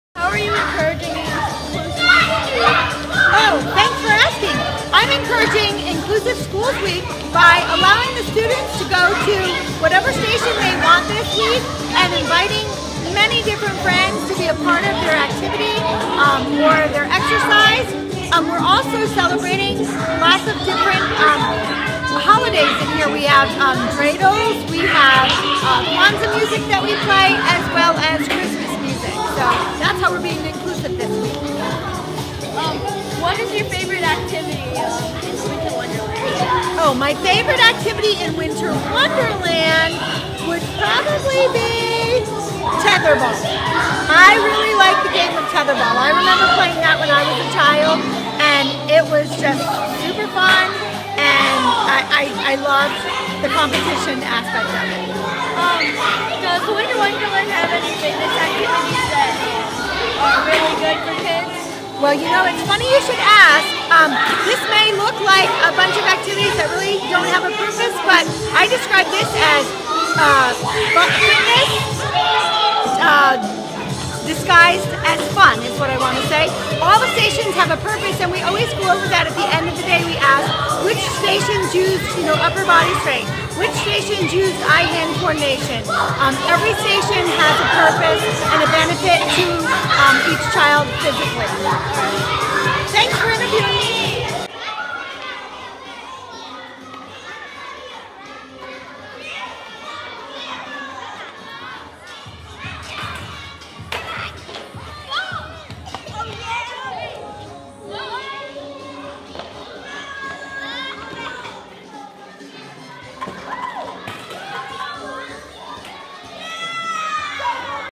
PE Winter Wonderland Interview with Ms. Swanson
Students have been enjoying the Winter Wonderland in PE!